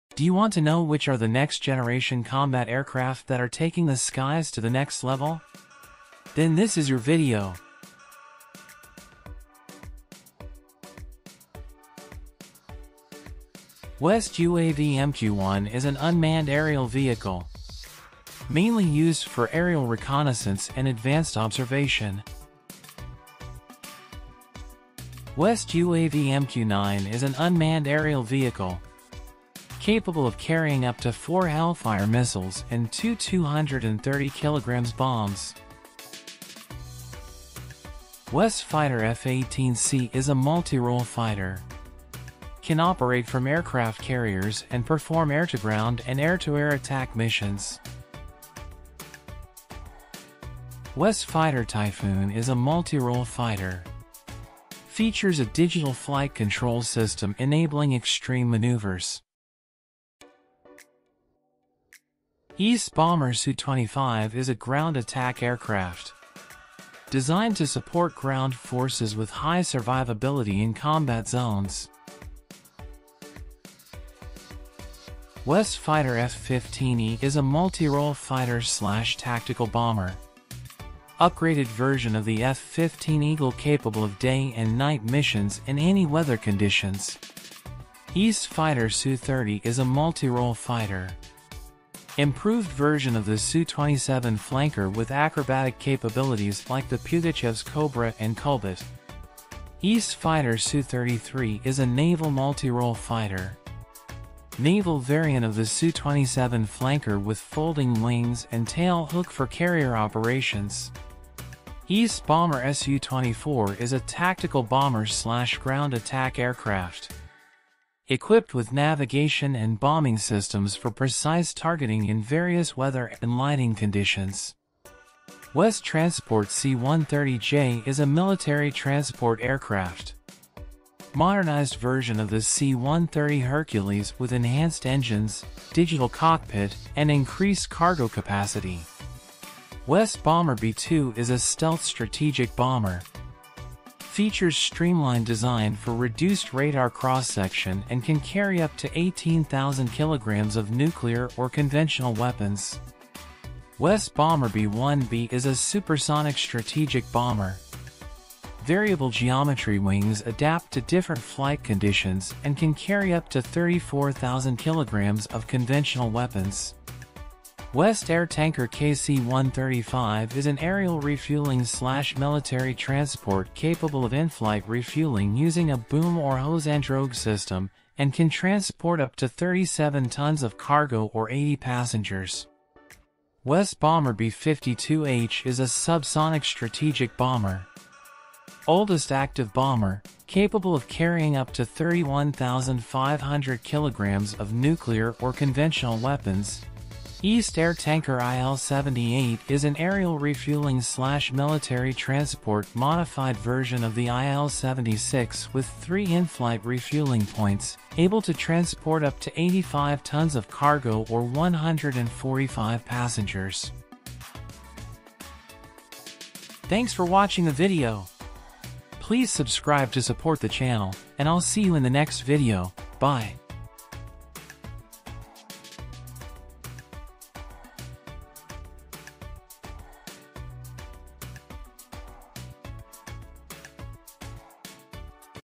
COMBAT AIRCRAFT
Greetings, technology and aviation enthusiasts! In this exciting video, we will take you on a journey into the SKY to explore the thrilling world of next-generation combat aircraft. From STEALTHY PLANES, CARGO AIRCRAFT, to cutting-edge DRONES, we will delve into the innovations that are shaping the future of air power.